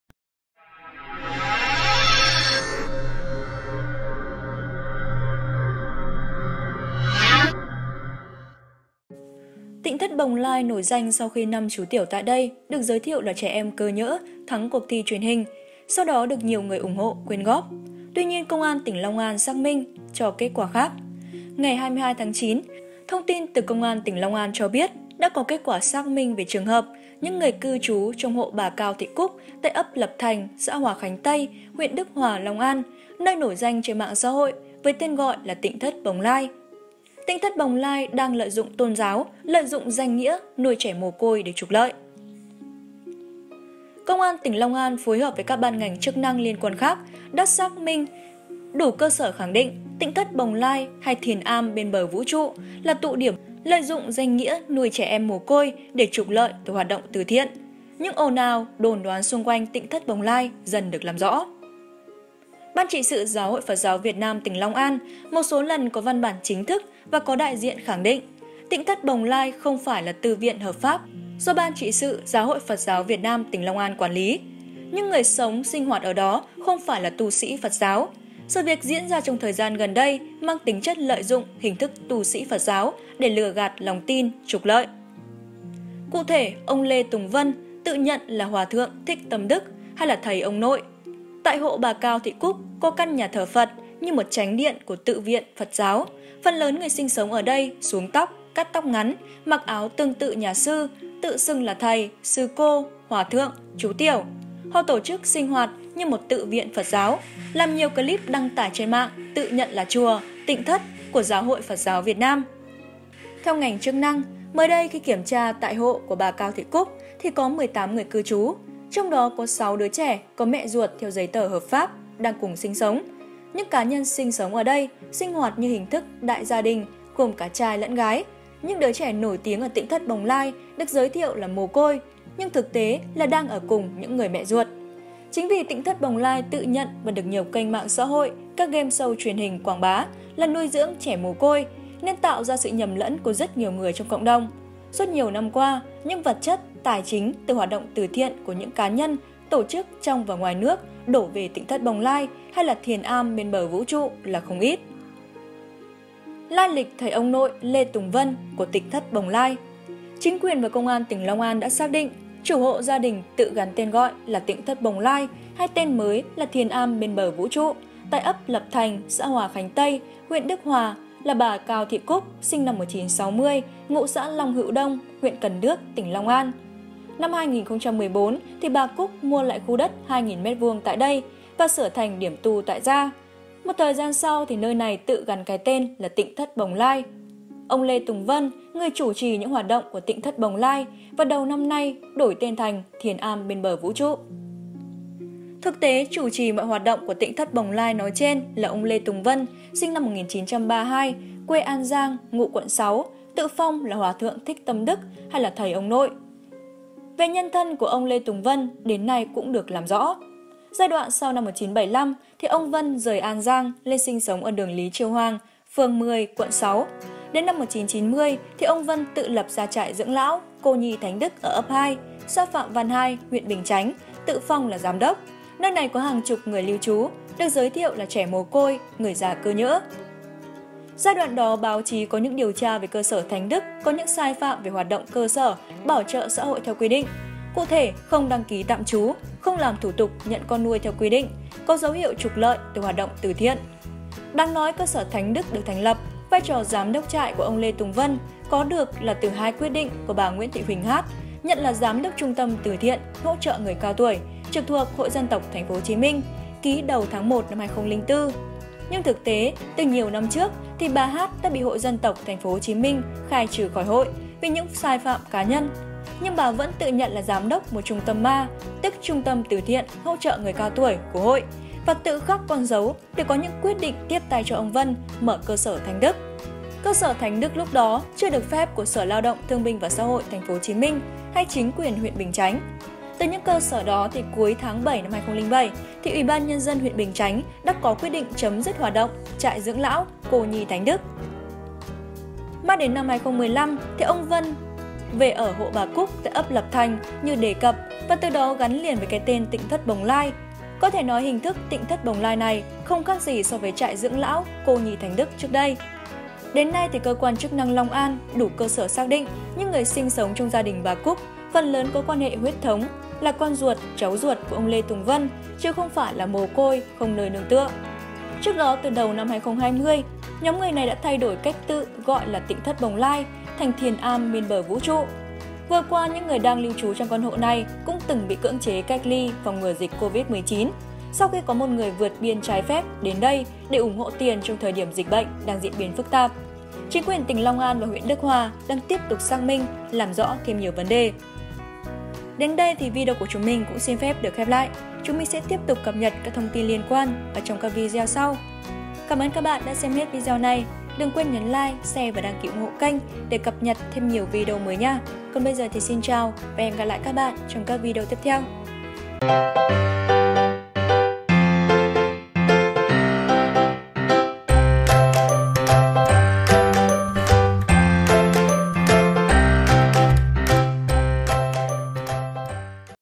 Tịnh thất Bồng Lai: trẻ mồ côi là con cháu ruột, lấy danh từ thiện để trục lợi?
'Tịnh thất Bồng Lai' nổi danh sau khi '5 chú tiểu' tại đây được giới thiệu là trẻ em cơ nhỡ thắng cuộc thi truyền hình, sau đó được nhiều người ủng hộ, quyên góp. Tuy nhiên Công an tỉnh Long An xác minh cho kết quả khác.
Ngày 22-9, thông tin từ Công an tỉnh Long An cho biết đã có kết quả xác minh về trường hợp những người cư trú trong hộ bà Cao Thị Cúc tại ấp Lập Thành, xã Hòa Khánh Tây, huyện Đức Hòa, Long An, nơi nổi danh trên mạng xã hội với tên gọi "Tịnh thất Bồng Lai".
"Tịnh thất Bồng Lai" đang Lợi dụng tôn giáo, lợi dụng danh nghĩa nuôi trẻ mồ côi để trục lợi
Công an tỉnh Long An phối hợp với các ban ngành chức năng liên quan khác đã xác minh, đủ cơ sở khẳng định, “Tịnh thất Bồng Lai” hay “Thiền am bên bờ vũ trụ” là tụ điểm lợi dụng tôn giáo, lợi dụng danh nghĩa nuôi trẻ mồ côi để trục lợi từ hoạt động từ thiện. Những ồn ào, đồn đoán xung quanh “Tịnh thất Bồng Lai” dần được làm rõ.
Ban trị sự Giáo hội Phật giáo Việt Nam tỉnh Long An một số lần có văn bản chính thức và có đại diện khẳng định, "Tịnh thất Bồng Lai" không phải là tự viện hợp pháp do Ban trị sự Giáo hội Phật giáo Việt Nam tỉnh Long An quản lý.
Những người sống, sinh hoạt ở đó không phải là tu sĩ Phật giáo. Sự việc diễn ra trong thời gian gần đây mang tính chất lợi dụng hình thức tu sĩ Phật giáo để lừa gạt lòng tin, trục lợi.
Cụ thể, ông Lê Tùng Vân tự nhận là Hoà thượng Thích Tâm Đức hay “thầy ông nội”. Tại hộ bà Cao Thị Cúc có căn thờ Phật như một chánh điện của tự viện Phật giáo. Phần lớn người sinh sống ở đây xuống tóc, cắt tóc ngắn, mặc áo tương tự nhà sư, tự xưng là thầy, sư cô, hoà thượng, chú tiểu… Họ tổ chức sinh hoạt như một tự viện Phật giáo; làm nhiều clip đăng tải trên mạng tự nhận là chùa, tịnh thất của Giáo hội Phật giáo Việt Nam.
Theo ngành chức năng, mới đây khi kiểm tra tại hộ bà Cao Thị Cúc thì có 18 người cư trú. Trong đó có 6 đứa trẻ có mẹ ruột theo giấy tờ hợp pháp, đang cùng sinh sống. Những cá nhân sinh sống ở đây, sinh hoạt như hình thức đại gia đình gồm cả trai lẫn gái. Những đứa trẻ nổi tiếng ở "Tịnh thất Bồng Lai" được giới thiệu là mồ côi nhưng thực tế là đang ở cùng những người mẹ ruột
Chính vì “Tịnh thất Bồng Lai” tự nhận và được nhiều kênh mạng xã hội, các games show truyền hình quảng bá là nuôi dưỡng trẻ mồ côi nên tạo ra sự nhầm lẫn của rất nhiều người trong cộng đồng. Suốt nhiều năm qua, những vật chất, tài chính từ hoạt động từ thiện của những cá nhân, tổ chức trong và ngoài nước đổ về “Tịnh thất Bồng Lai” hay “Thiền am bên bờ vũ trụ” là không ít.

Lai lịch “thầy ông nội” Lê Tùng Vân của “Tịnh thất Bồng Lai”
Chính quyền và Công an tỉnh Long An đã xác định, chủ hộ gia đình tự gắn tên gọi là “Tịnh thất Bồng Lai” hay tên mới “Thiền am bên bờ vũ trụ” tại ấp Lập Thành, xã Hoà Khánh Tây, huyện Đức Hoà, là bà Cao Thị Cúc (SN 1960, ngụ xã Long Hựu Đông, huyện Cần Đước, tỉnh Long An).
Năm 2014, bà Cúc mua lại khu đất 2.000m2 tại đây và sửa thành điểm tu tại gia. Một thời gian sau, nơi này tự gắn cái tên “Tịnh thất Bồng Lai”.
Ông Lê Tùng Vân, người chủ trì những hoạt động của "Tịnh thất Bồng Lai" và đầu năm nay đổi tên thành "Thiền am bên bờ vũ trụ"